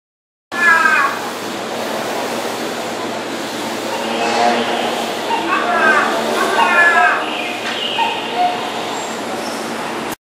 ９/８ カラス君の朝の挨拶
通りゆく子達を録っている私の頭上で「おはー」と鳴き始めましたので走って来る子をそっちのけでカラス君を録ってみました(笑）